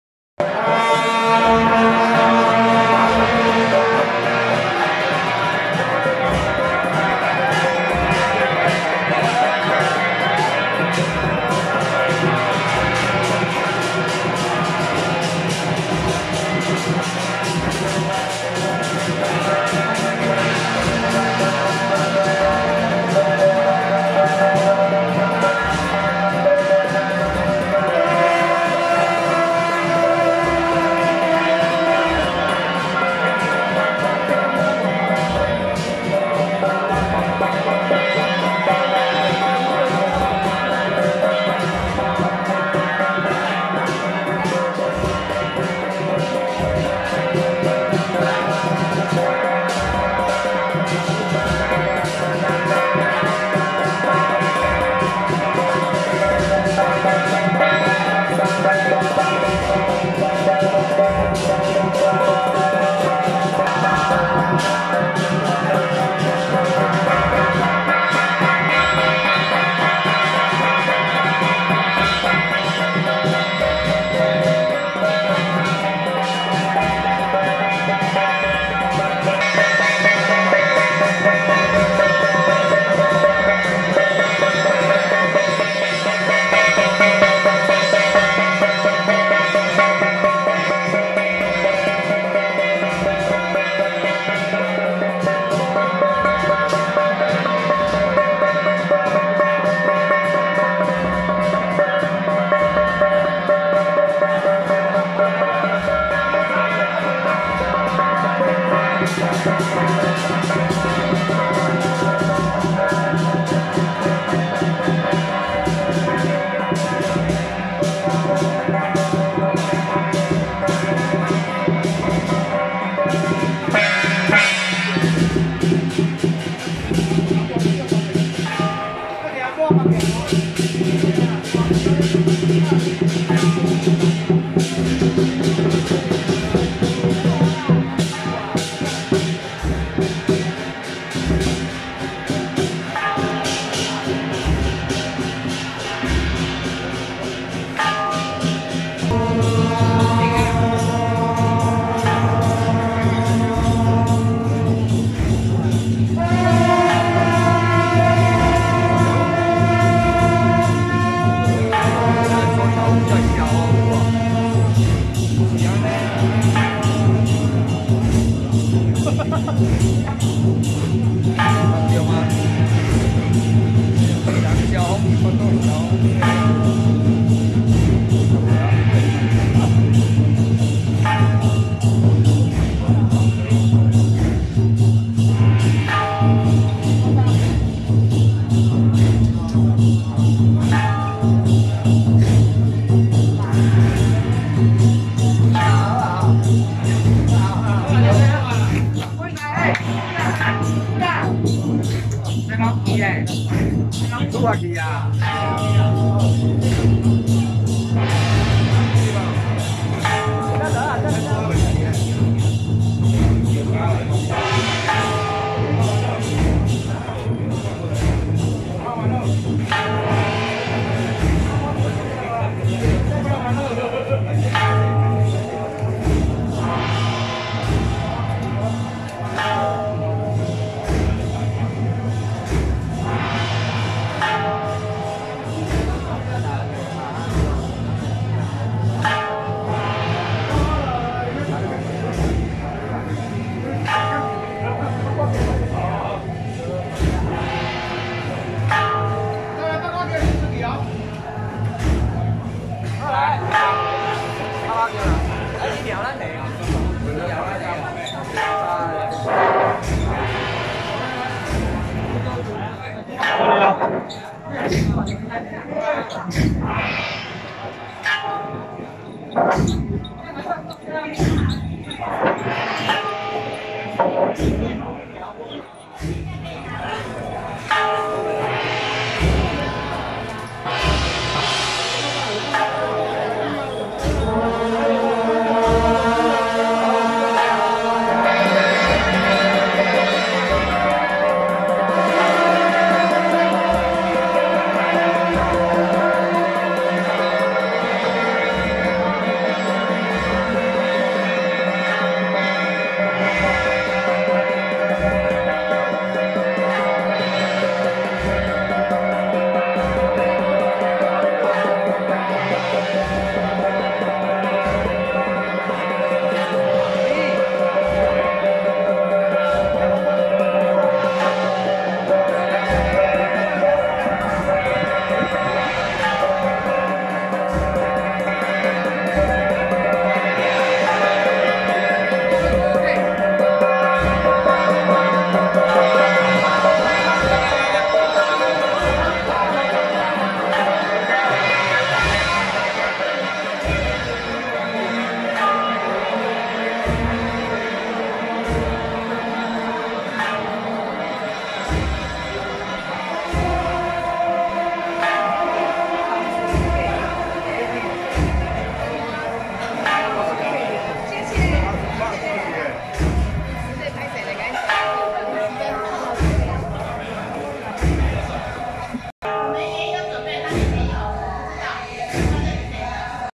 【回鑾影片】~109大甲聖法宮鄭府三元帥,列位尊神往中和霹靂宮進香
(0:00)大甲鎮瀾宮-哨角隊
(1:35)大甲聖法宮-報馬仔
(1:47)大甲聖天龍獅團-醒獅團
(2:29)大甲頂店頂安宮-中壇元帥、濟公禪師
(3:22)千順將軍
(4:18)大甲聖法宮-列位尊神 聖駕
(4:58)大甲聖法宮-鄭府三元帥,列位尊神 聖駕